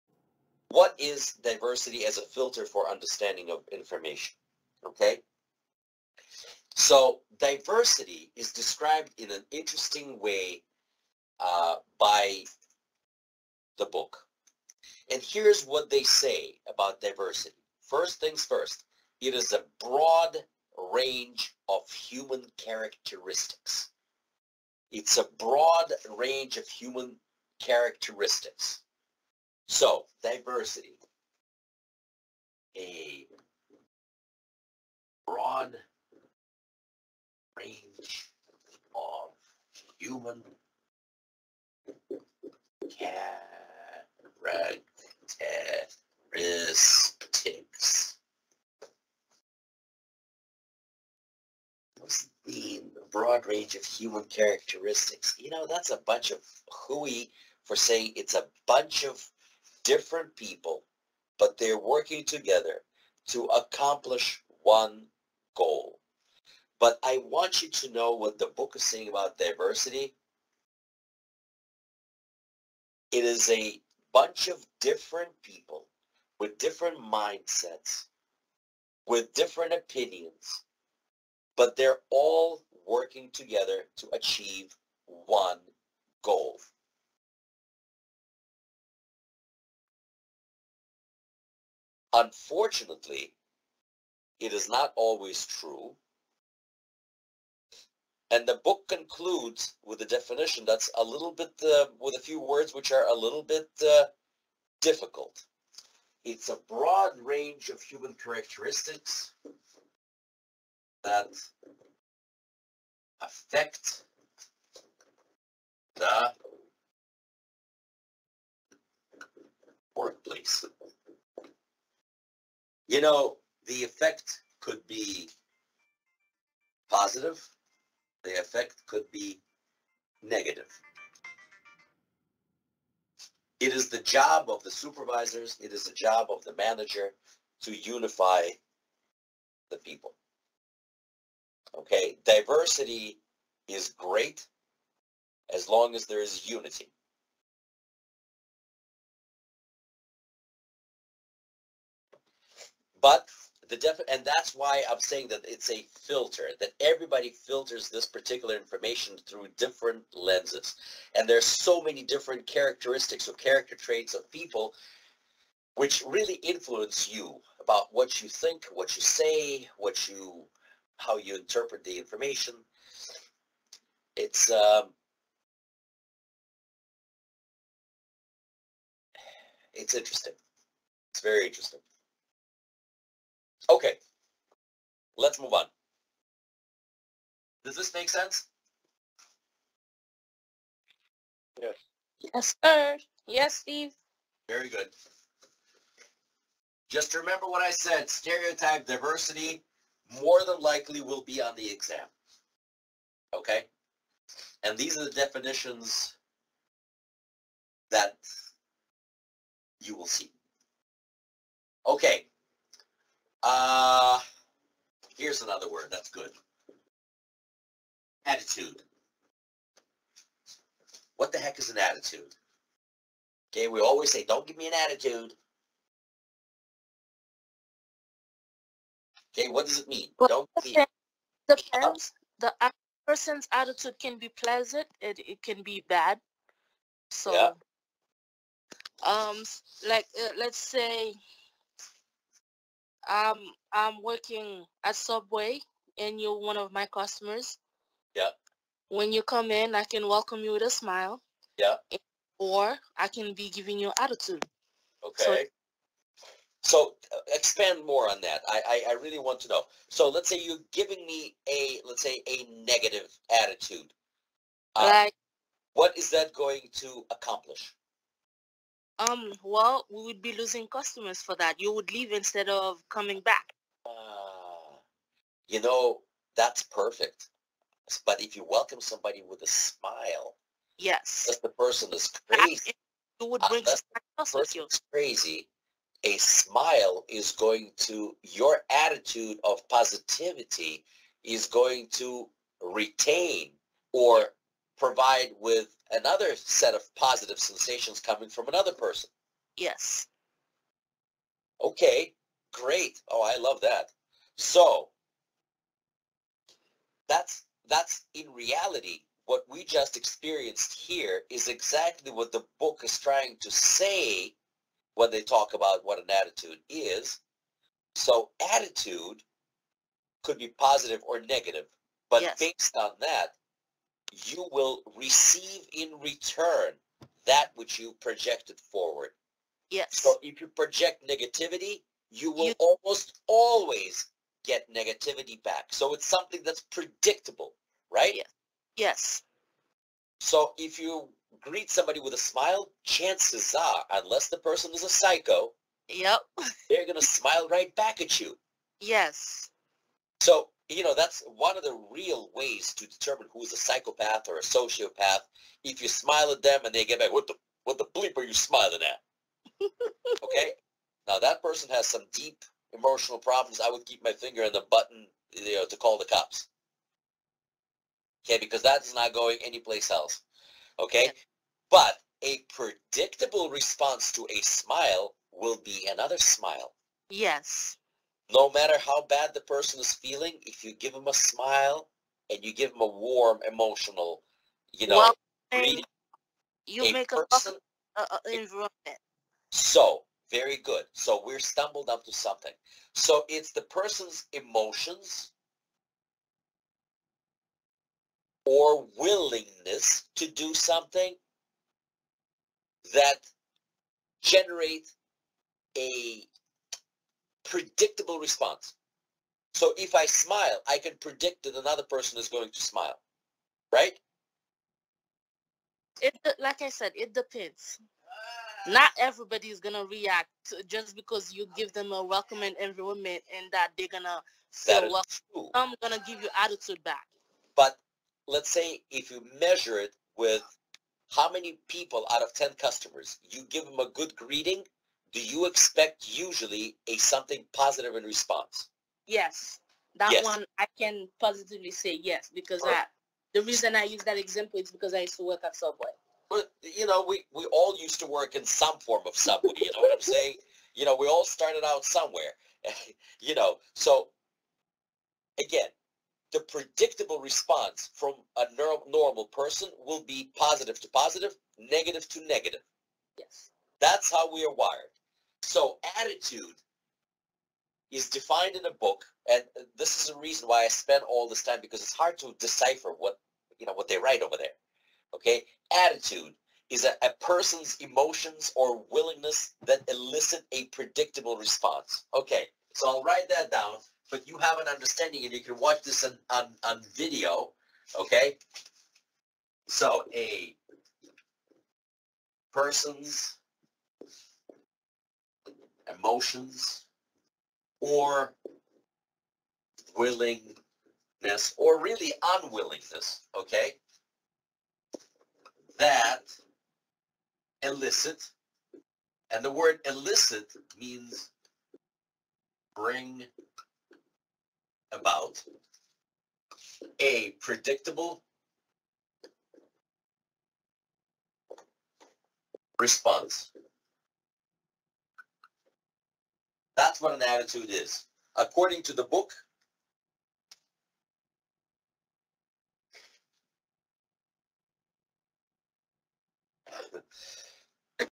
Diversity and Attitude
Steve Yanovsky explains topics related to human communications and relations as they pertain to the CRCST examination by the HSPA. In this clip we discuss diversity and attitude.